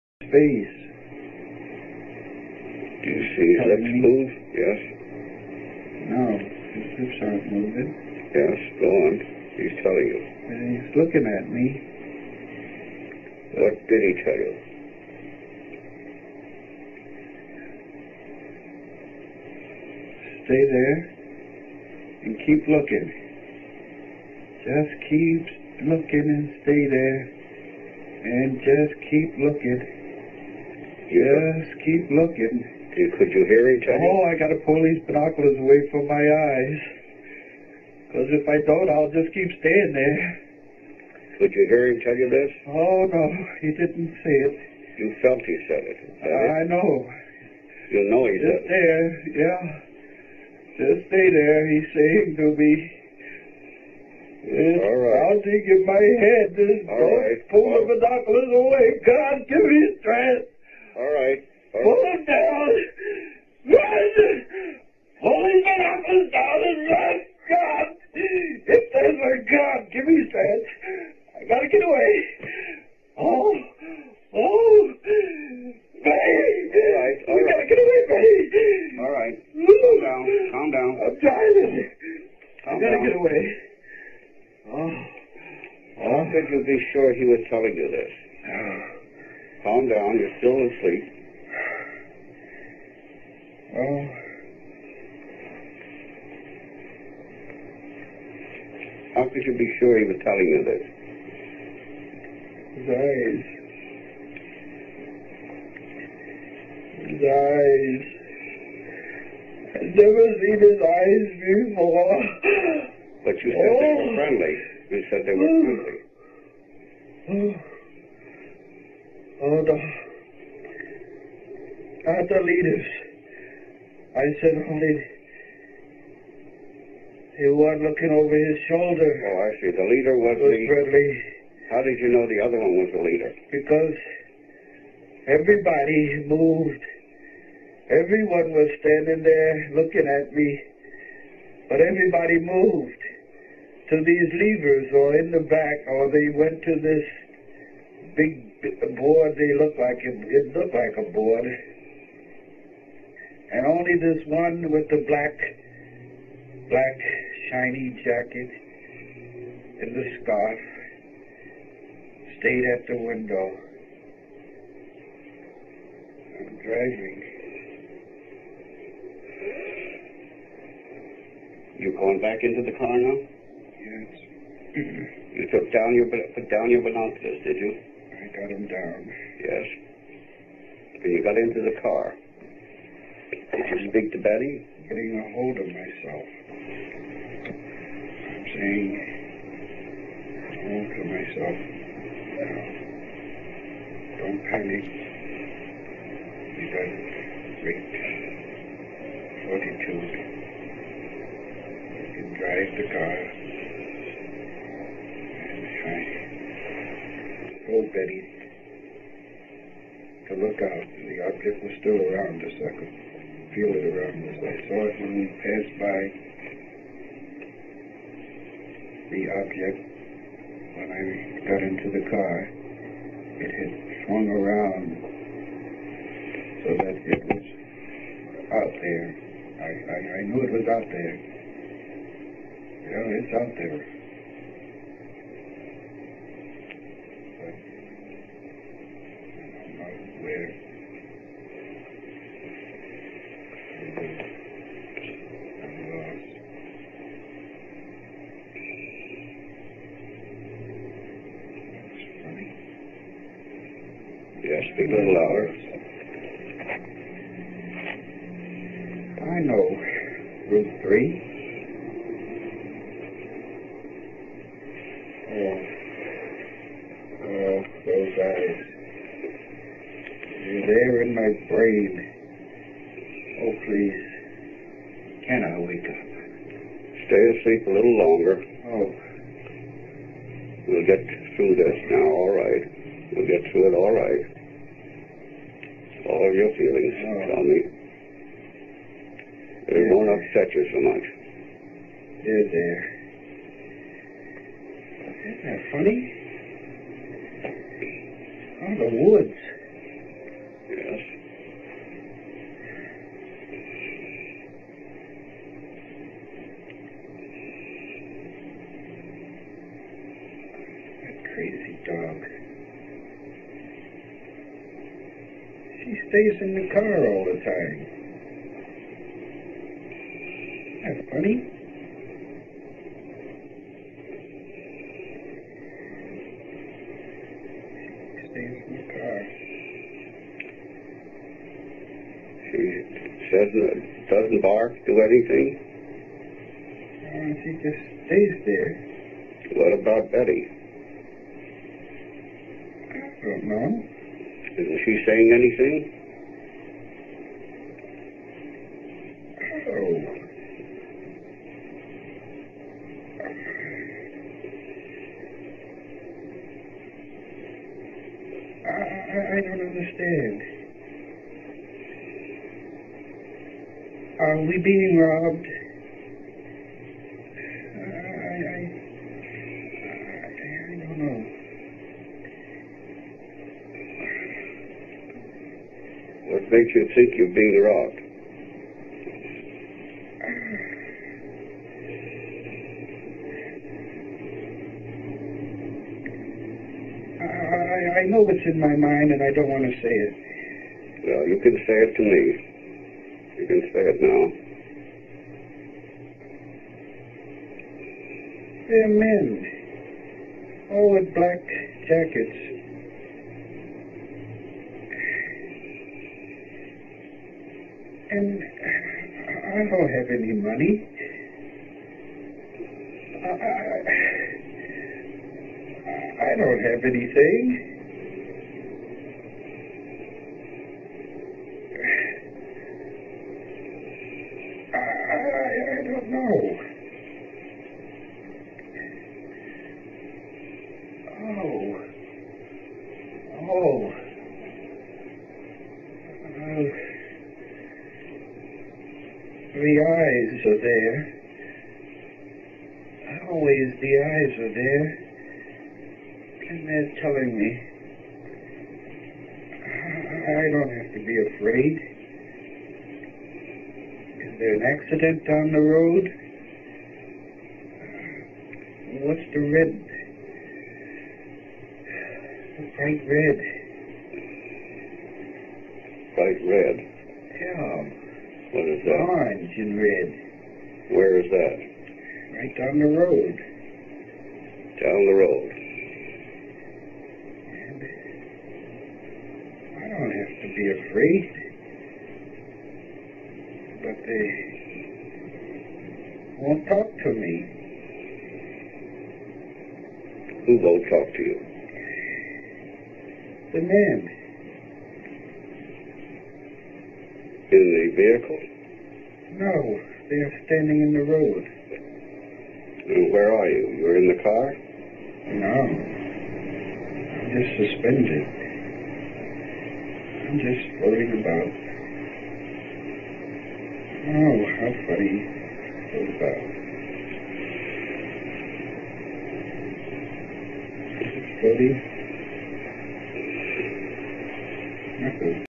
UFO Abduction-Barney Hill Hypnosis Session 3 of 5
Betty and Barney Hill had an abduction experience that occurred on September 19,1961 in the White Mountains area of New Hampshire.This is the original hypnosis session tape of Barney Hill who is being questioned by Dr. Benjamin Simons.